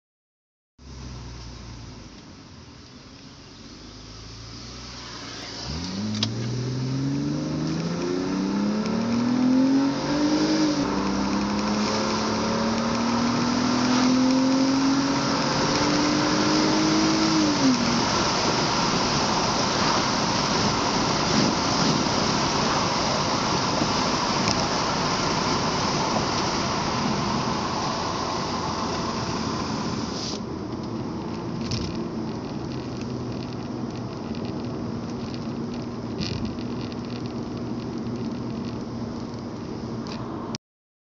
BMW E46 325i intake full throttle
Custom built intake in BMW E46 325i Window's down full acceleration